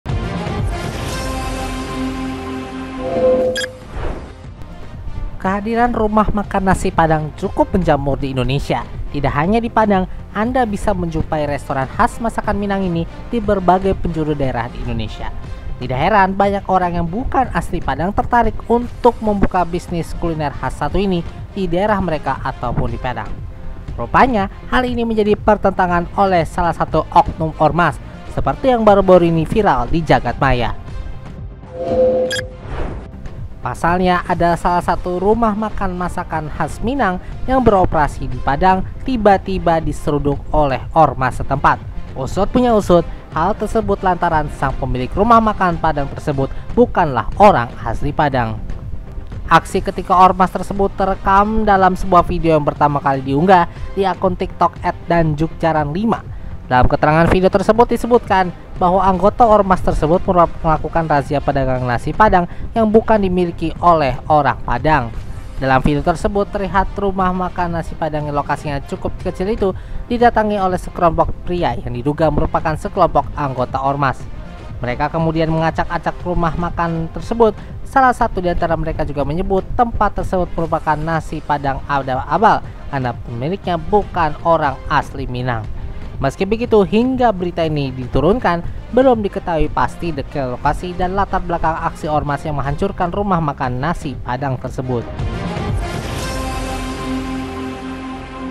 Ormas Razia Rumah Makan Padang Harga Merakyat yang Pemiliknya bukan Orang Padang - NP Evening News
Situasi tak biasa baru terjadi Cirebon karena beberapa rumah makan Padang dirazia oleh grup PRMPC alias Perhimpunan Rumah Makan Padang Cirebon.
Mereka melakukan 'sweeping' di kota Udang tersebut dan melakukan pencobotan label tulisan 'masakan Padang' rumah makan Minang yang penjualnya non-Sumbar.